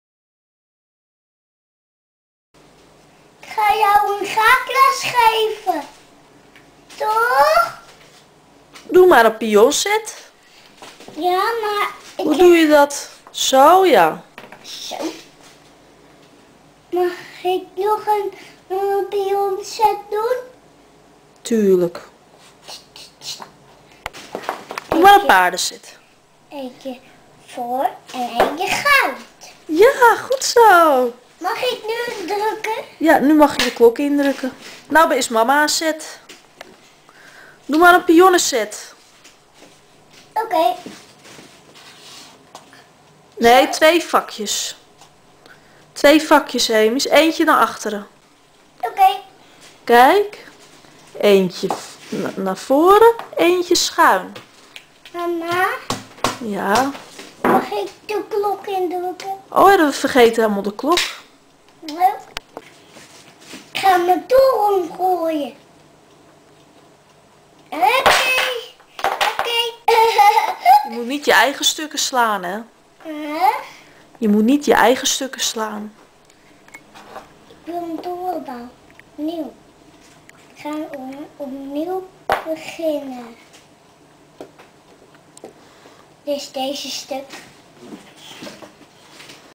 Hamish wordt later grootmeester
Hij is 3 en hij weet al hoe je een pion en paard moet zetten.